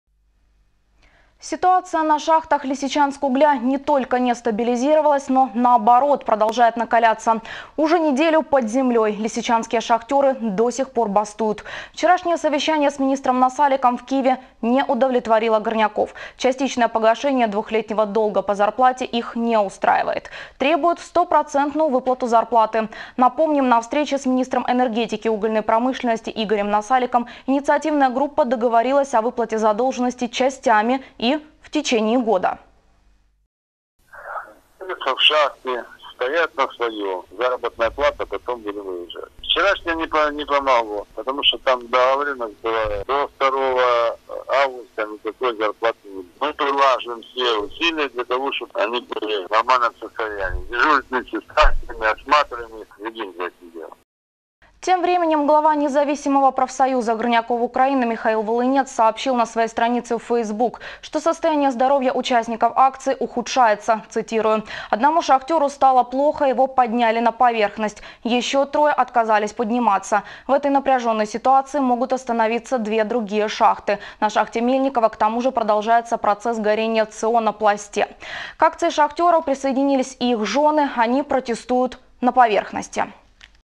Ситуация на шахтах «Лисичанскугля» продолжает накаляться
Ситуация на шахтах «Лисичанскугля» не только не стабилизировалась, но, наоборот, продолжает накаляться. Уже неделю под землей. Лисичанские шахтеры до сих пор бастуют. Вчерашнее совещание с министром Насаликом в Киеве не удовлетворило горняков.